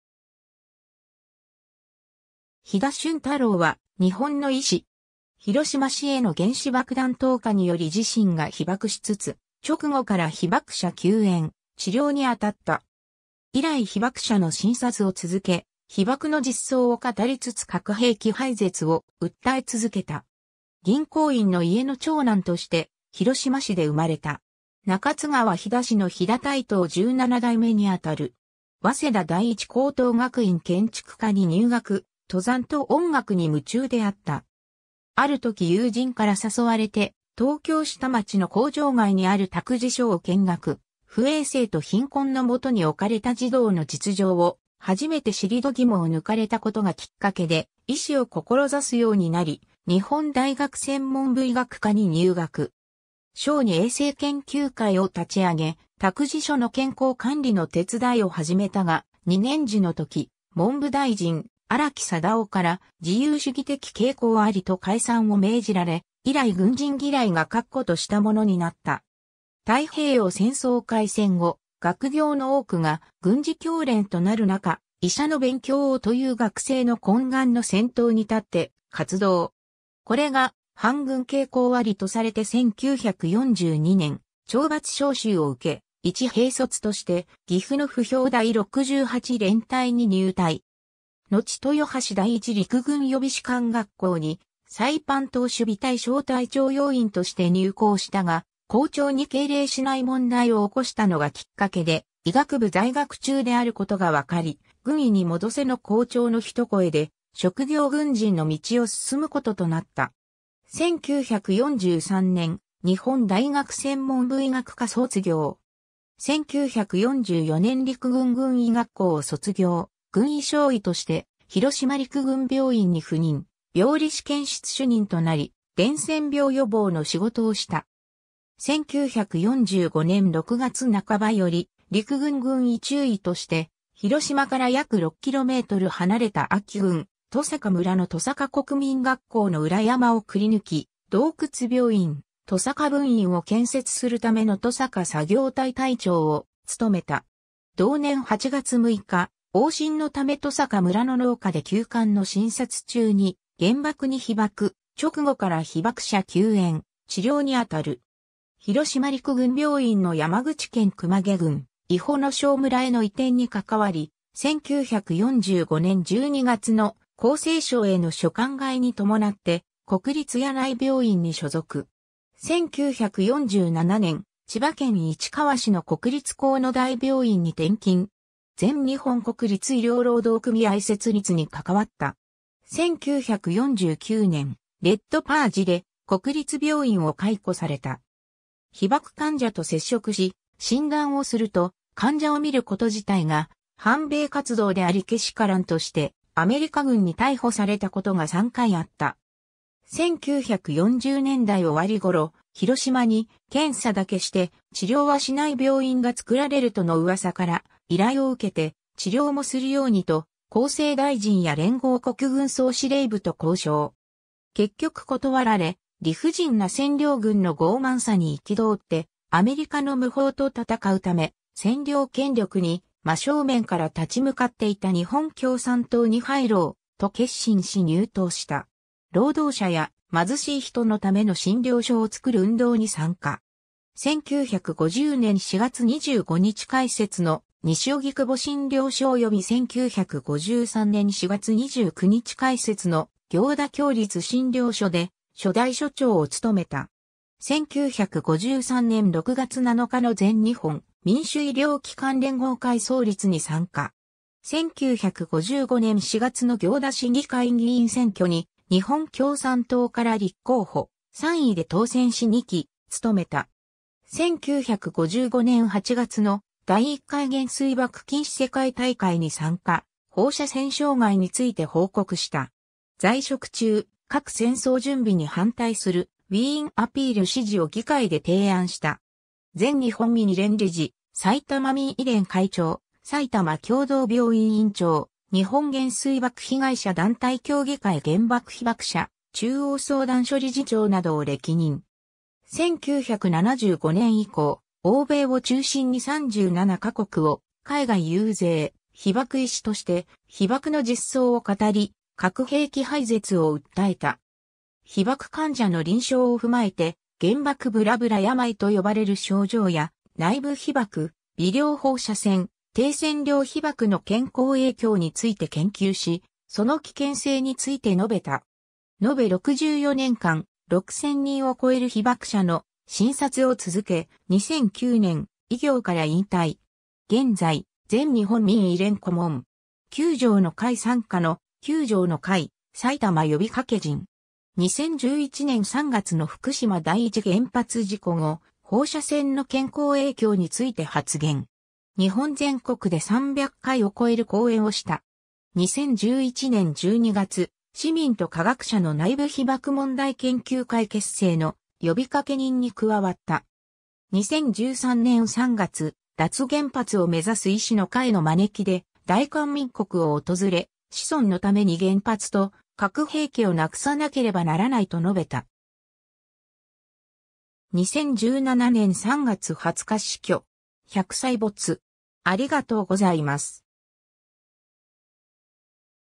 肥田舜太郎
国立精神・神経医療研究センターの人物
肥田 舜太郎（ひだ しゅんたろう、1917年1月1日 -2017年3月20日）は、日本の医師。
広島市への原子爆弾投下により自身が被爆しつつ、直後から被爆者救援・治療にあたった。
以来被爆者の診察を続け、被爆の実相を語りつつ核兵器廃絶を訴え続けた。
 銀行員の家の長男として広島市で生まれた。
中津川肥田氏の肥田帯刀17代目にあたる。
早稲田第一高等学院建築科に入学、登山と音楽に夢中であった。
あるとき友人から誘われて東京下町の工場街にある託児所を見学、不衛生と貧困のもとにおかれた児童の実情を初めて知り度肝をぬかれたことがきっかけで、医師を志すようになり、日本大学専門部医学科に入学。
「小児衛生研究会」を立ち上げ、託児所の健康管理の手伝いを始めたが、2年次のとき文部大臣荒木貞夫から「自由主義的傾向あり」と解散を命じられ、以来軍人嫌いが確固としたものになった。
太平洋戦争開戦後、学業の多くが軍事教練となる中「医者の勉強を」という学生の懇願の先頭に立って活動。
これが「反軍傾向あり」とされて1942年懲罰召集を受け、一兵卒として岐阜の歩兵第68連隊に入隊。
のち豊橋第一陸軍予備士官学校にサイパン島守備隊小隊長要員として入校したが、校長に敬礼しない問題を起こしたのがきっかけで医学部在学中であることがわかり、「軍医に戻せ」の校長の一声で職業軍人の道を進むこととなった。
 1943年、日本大学専門部医学科卒業。
1944年陸軍軍医学校を卒業、軍医少尉として広島陸軍病院（現広島赤十字・原爆病院）に赴任、病理試験室主任となり伝染病予防の仕事をした。
1945年6月半ばより、陸軍軍医中尉として、広島から約6km離れた安芸郡戸坂村（現・同市東区戸坂）の戸坂国民学校（現・広島市立戸坂小学校）の裏山をくりぬき洞窟病院「戸坂分院」を建設するための戸坂作業隊隊長を務めた。
同年8月6日、往診のため戸坂村の農家で急患の診察中に原爆に被爆、直後から被爆者救援・治療にあたる。
広島陸軍病院の山口県熊毛郡伊保庄村（現柳井市）への移転にかかわり、1945年12月の厚生省への所管替えに伴って国立柳井病院（現国立病院機構柳井病院）に所属。
 1947年、千葉県市川市の国立国府台病院に転勤。
全日本国立医療労働組合設立にかかわった。
1949年、レッドパージで国立病院を解雇された。
 被爆患者と接触し診断をすると、患者を診ること自体が反米活動でありけしからんとして、アメリカ軍に逮捕されたことが3回あった。
1940年代終わり頃、広島に検査だけして治療はしない病院（原爆傷害調査委員会:ABCC）がつくられるとの噂から、依頼を受けて、治療もするようにと厚生大臣や連合国軍総司令部と交渉。
結局断られ、理不尽な占領軍の傲慢さに憤って、アメリカの無法と闘うため占領権力に真正面から立ち向っていた日本共産党に入ろう、と決心し入党した。
 労働者や貧しい人のための診療所をつくる運動に参加。
1950年4月25日開設の西荻窪診療所（東京都杉並区）および1953年4月29日開設の行田協立診療所（埼玉県行田市）で、初代所長を務めた。
1953年6月7日の全日本民主医療機関連合会（全日本民医連）創立に参加。
 1955年4月の行田市議会議員選挙（第3回統一地方選挙）に日本共産党から立候補、3位で当選 し2期務めた。
1955年8月の第一回原水爆禁止世界大会に参加、放射線障碍について報告した。
在職中「核戦争準備に反対するウィーン・アピール」支持を議会で提案した。
 全日本民医連理事、埼玉民医連会長、埼玉協同病院院長、日本原水爆被害者団体協議会原爆被爆者中央相談所理事長などを歴任。
1975年以降、欧米を中心に37か国 を海外遊説、被爆医師として被爆の実相を語り、核兵器廃絶を訴えた。
 被爆患者の臨床をふまえて、「原爆ぶらぶら病」とよばれる症状や、内部被曝、微量放射線・低線量被曝の健康影響について研究し、その危険性について述べた。
のべ64年間、6000人を超える被爆者の診察を続け、2009年、医業から引退。
 現在、全日本民医連顧問。
「九条の会」傘下の「九条の会・さいたま」呼びかけ人。
 2011年3月の福島第一原発事故後、放射線の健康影響について発言。
日本全国で300回を超える講演をした。
 2011年12月、市民と科学者の内部...